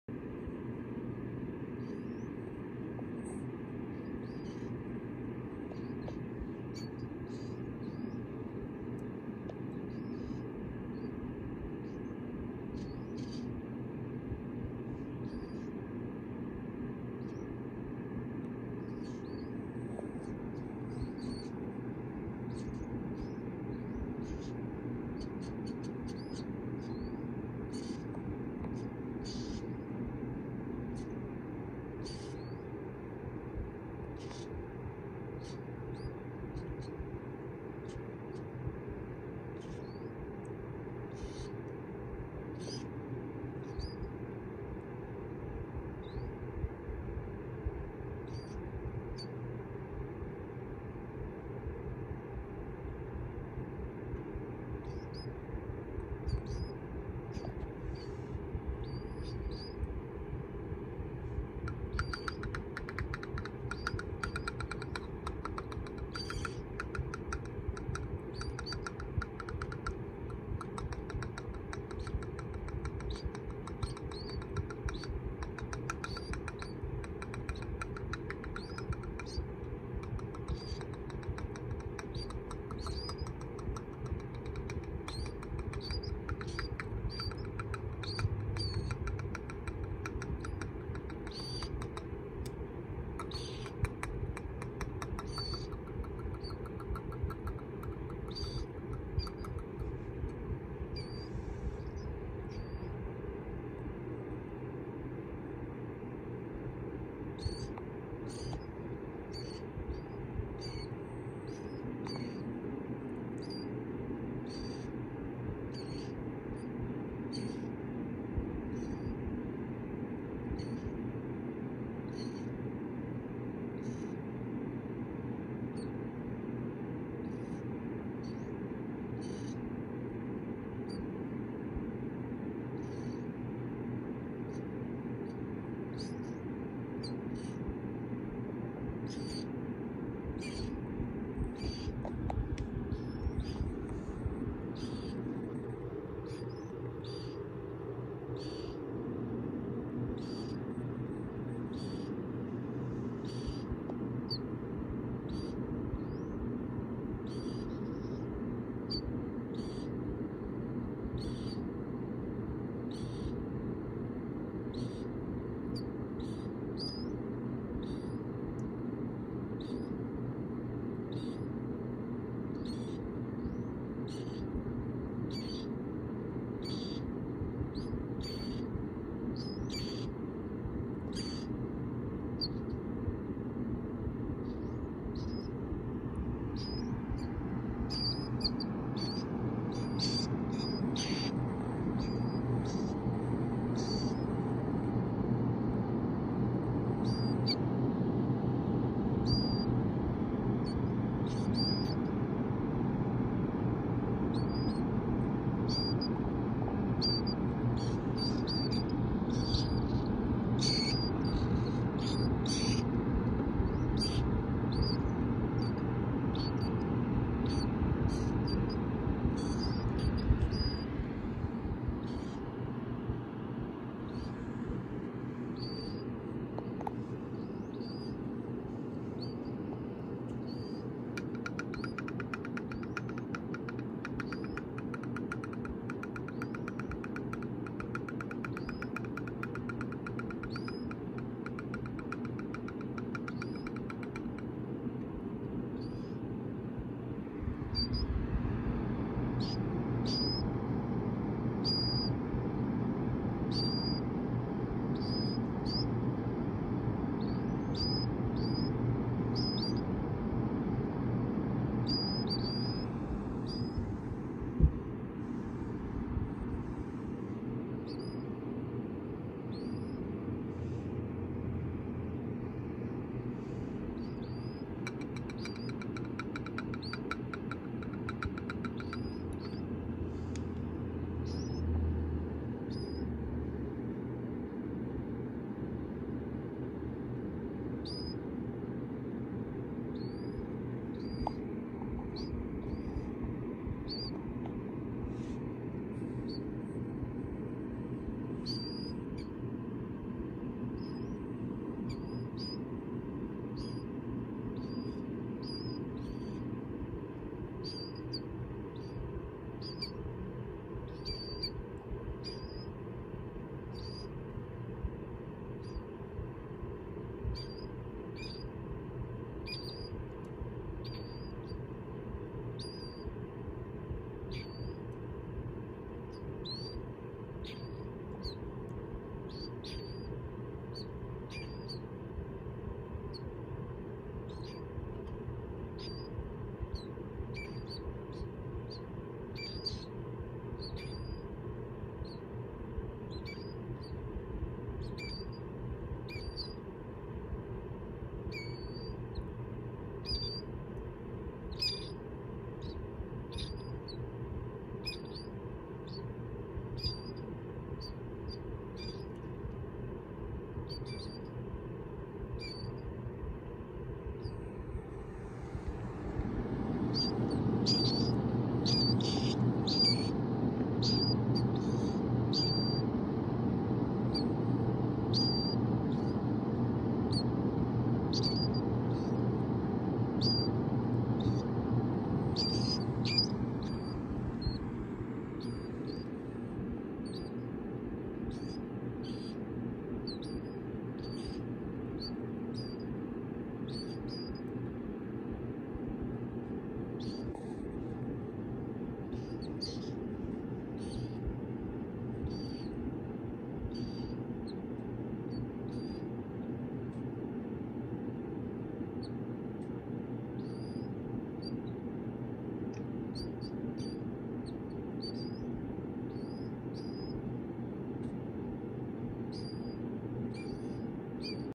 Rehabilitating Baby KillDeer
Someone thought they found some Quail eggs in their harvest fields right before spraying and took them to a friend who incubates chicken eggs and raises birds. When they came to hatch, they realized they were not Quail. They were baby Killdeer. So now they have to be rehabilitated and put back in the wild. Their diet consists of bugs, worms, larvae, fish, shrimp, and a few other insects as close to a normal environment. Electrolytes added to freshwater. A constant source of heat. If baby Killdeer do not have the food they can die within a couple of hours. 60-70% do not survive in the wild. Due to falls, predators, and starvation. Parents do not feed their young. Babies do not begin to fly for 30 days after hatching. But are still protected and stay close to parents. They leave the nest within 24 hrs of hatching and search for food immediately. Always know what nest, feather, bird, egg you come across before you decide to pick it and “rescue” it. Some and most are protected.